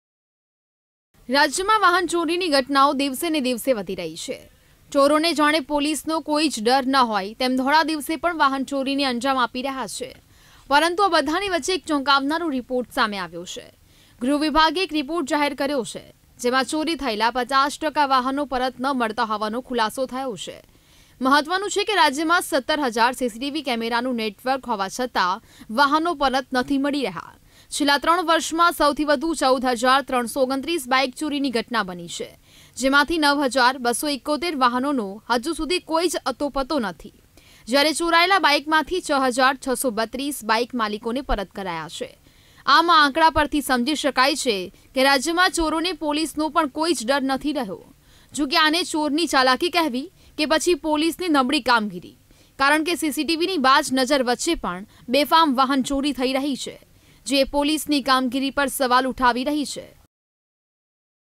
Jamnagar News: રાજ્યમાં બાઈક ચોરો બેફામ બન્યાં, આ બધાની વચ્ચે એક ચોંકાવનારો રિપોર્ટ સામે આવ્યો
Jamnagar News: રાજ્યમાં વાહન ચોરીની ઘટનાઓ દિવસે દિવેસ વધી રહી છે.,ચોરોને જાણે પોલીસનો કોઈ જ ડર ન હોય તેમ ધોળા દિવસે પણ વાહન ચોરીને અંજામ આપી રહ્યાં છે. | VTV GUJARATI